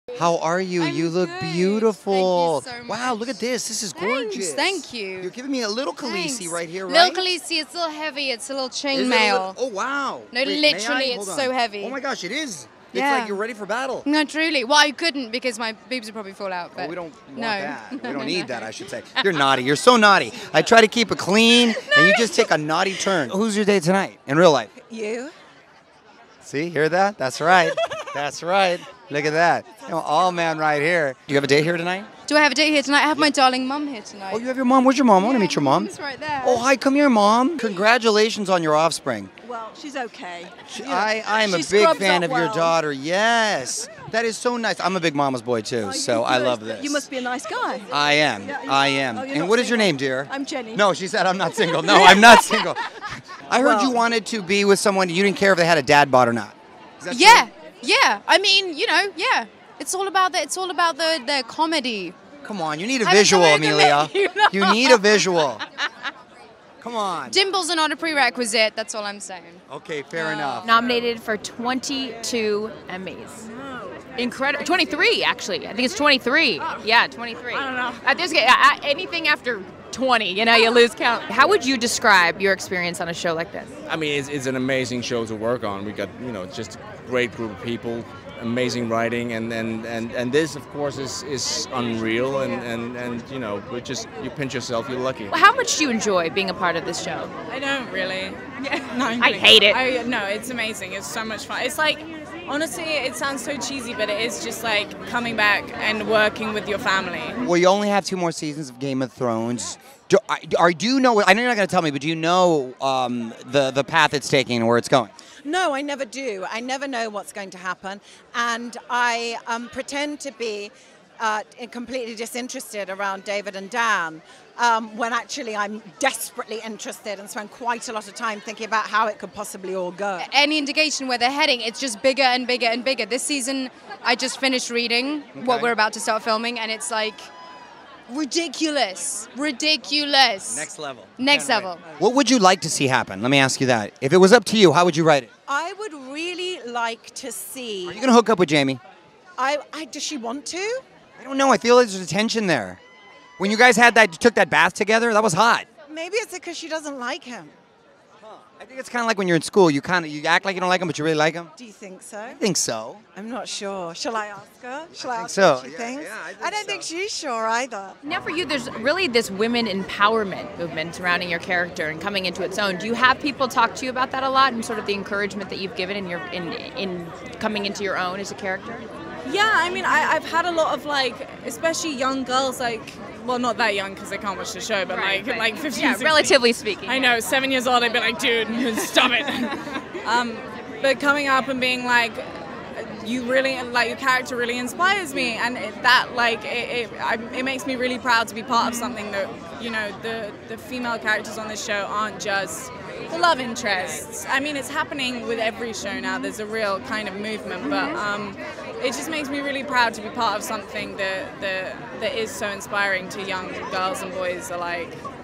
Emilia Clarke Jokes That Emmys Dress Wouldn't Work on ‘Thrones’: ‘My Boobs Would Probably Fall Out’
“Extra” caught up with the cast of “Game of Thrones” on the red carpet, ahead of the show’s big Emmy win for Outstanding Drama.

Watch the video to see Emilia Clarke joke about her gorgeous gown, revealing her character Khaleesi could never wear it on the show even though it is made of chain mail. “My boobs would probably fall out!”

We also dished with Emilia’s co-stars about what is coming up next season. Watch!

Who will rule the Iron Throne? Leave your predictions in the comments.